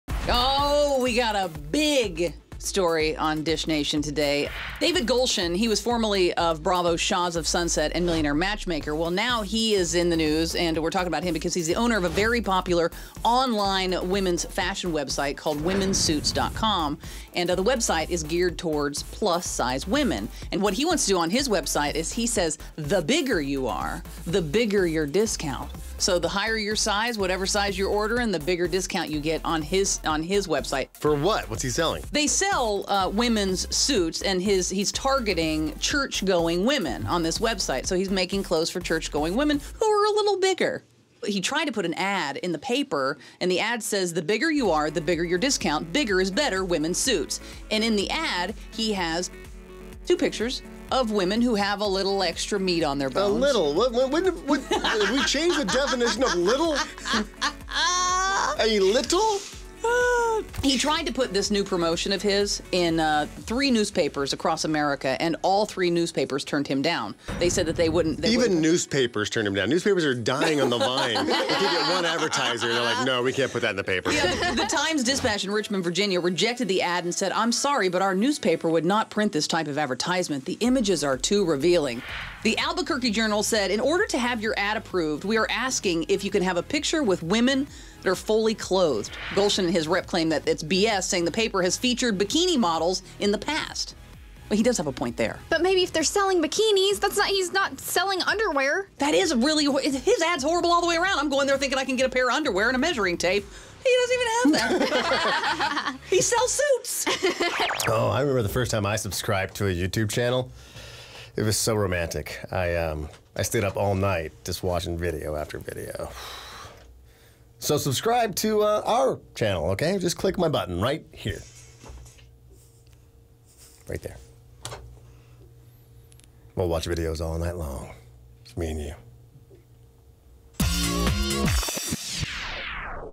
Reality TV Star Cries Discrimination Over His Plus-Size Ads
David Golshan, former star of Bravo’s Shahs of Sunset and Millionaire Matchmaker, is crying discrimination over an ad for his popular women’s fashion website, Womensuits.com, after several news outlets refused to run it.

Golshan and his rep claim it’s because the colorful ad features plus-size women, Dish Nation is exclusively reporting.